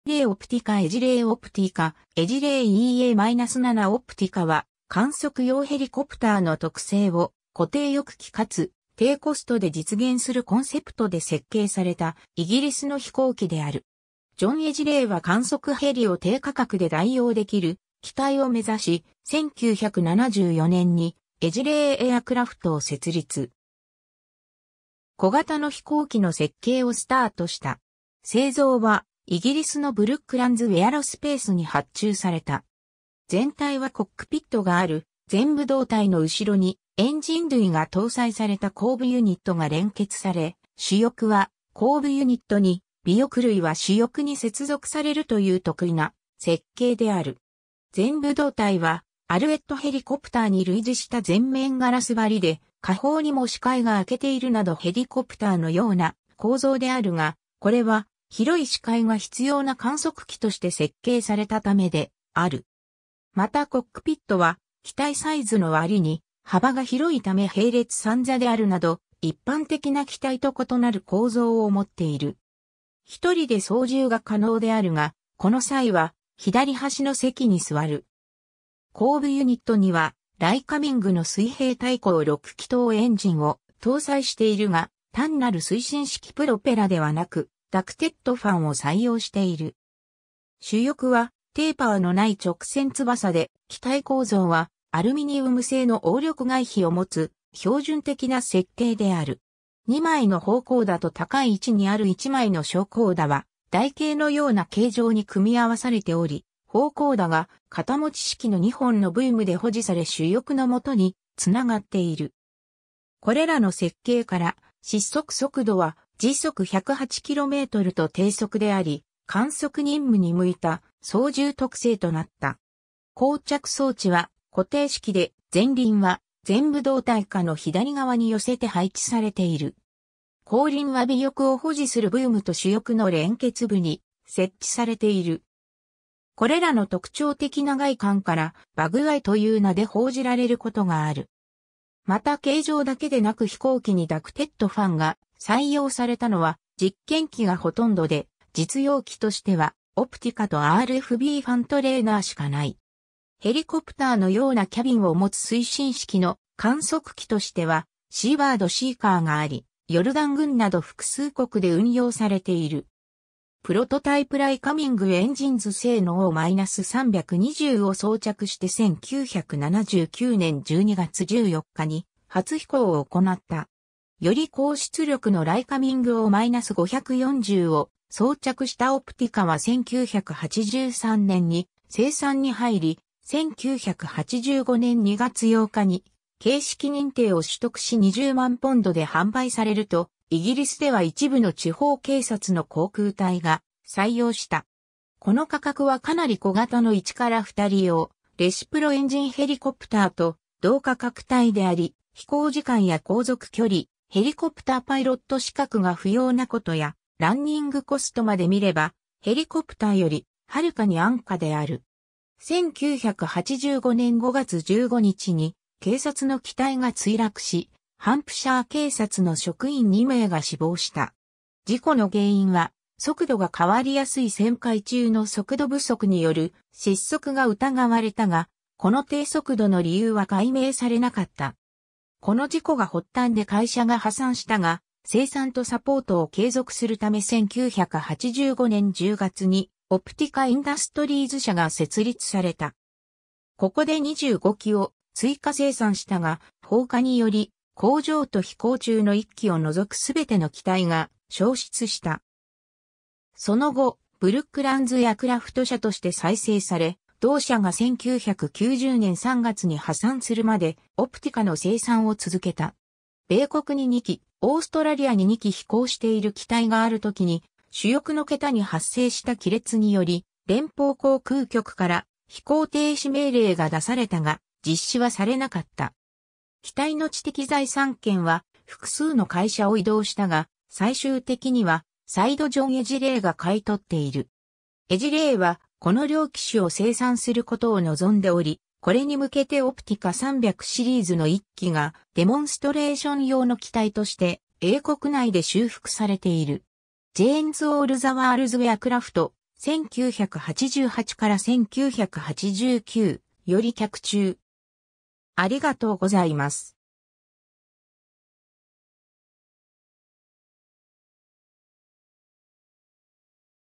エジレイ オプティカ
エジレイ オプティカ エジレイ オプティカ エジレイ EA-7 オプティカ（Edgley EA-7 Optica）は、観測用ヘリコプターの特性を、固定翼機かつ、低コストで実現するコンセプトで設計されたイギリスの飛行機である。
 ジョン・エジレイ（John Edgley）は観測ヘリを低価格で代用できる機体を目指し、1974年にエジレイ・エアクラフトを設立。
小型の飛行機の設計をスタートした。
製造はイギリスのブルックランズ・エアロスペースに発注された。
 全体はコックピットがある前部胴体の後ろにエンジン類が搭載された後部ユニットが連結され、主翼は後部ユニットに、尾翼類は主翼に接続されるという特異な設計である。
 前部胴体はアルエット ヘリコプターに類似した前面ガラス張りで下方にも視界が開けているなどヘリコプターの様な構造であるが、これは広い視界が必要な観測機として設計されたためである。
またコックピットは機体サイズの割に幅が広いため並列3座であるなど一般的な機体と異なる構造をもっている。
一人で操縦が可能であるが、この際は左端の席に座る。
 後部ユニットにはライカミングの水平対向6気筒エンジンを搭載しているが、単なる推進式プロペラではなくダクテッドファンを採用している。
 主翼はテーパーの無い直線翼で、機体構造はアルミニウム製の応力外皮を持つ標準的な設計である。
2枚の方向舵と高い位置にある1枚の昇降舵は台形のような形状に組み合わされており、方向舵が片持ち式の2本のブームで保持され主翼の下に繋がっている。
これらの設計から失速速度は108 km/hと低速であり、観測任務に向いた操縦特性となった。
 降着装置は固定式で、前輪は前部胴体下の左側に寄せて配置されている。
後輪は尾翼を保持するブームと主翼の連結部に設置されている。
 これらの特徴的な外観から「バグ・アイ」（"bug-eye"、虫眼）という名で報じられることがある。
 また形状だけでなく飛行機にダクテッドファンが採用されたのは実験機がほとんどで、実用機としてはオプティカとRFB ファントレーナーしかない。
 ヘリコプターの様なキャビンを持つ推進式の観測機としてはシーバード・シーカーがあり、ヨルダン軍など複数国で運用されている。
 プロトタイプ ライカミング・エンジンズ製のO-320を装着して1979年12月14日に初飛行を行った。
より高出力のライカミング O-540を装着したオプティカは1983年に生産に入り、1985年2月8日に型式認定を取得し20万ポンド（当時）で販売されると、イギリスでは一部の地方警察の航空隊が採用した。
この価格はかなり小型の1〜2人用レシプロ（ピストン）エンジンヘリコプターと同価格帯であり、飛行時間や航続距離、ヘリコプターパイロット資格が不要なことや、ランニングコストまでみれば、ヘリコプターより遥かに安価である。
 1985年5月15日に警察の機体（G-KATY）が墜落し、ハンプシャー警察の職員2名が死亡した。
事故の原因は速度が変わりやすい旋回中の速度不足による失速が疑われたが、この低速度の理由は解明されなかった。
この事故が発端で会社が破産したが、生産とサポートを継続するため1985年10月にオプティカ・インダストリーズ社が設立された。
ここで25機を追加生産したが、放火により工場と飛行中の1機を除く全ての機体が焼失した。
その後、ブルックランズ・エアクラフト社（Brooklands Aircraft）として再生され、同社が1990年3月に破産するまでオプティカの生産を続けた。
 米国に2機、オーストラリアに2機飛行している機体があるときに主翼の桁に発生した亀裂により連邦航空局（FAA）から飛行停止命令が出されたが実施はされなかった。
 機体の知的財産権は複数の会社を移動したが、最終的に（FLS スプリント160の設計と共に）は再度ジョン・エジレイが買い取っている。
エジレイはこの両機種を生産することを望んでおり、これに向けてオプティカ300シリーズの1機（シリアルナンバー：021 G-BOPO）がデモンストレーション用の機体として英国内で修復されている。
 （オプティカ スカウト）Jane's All the World's Aircraft, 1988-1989 より 脚注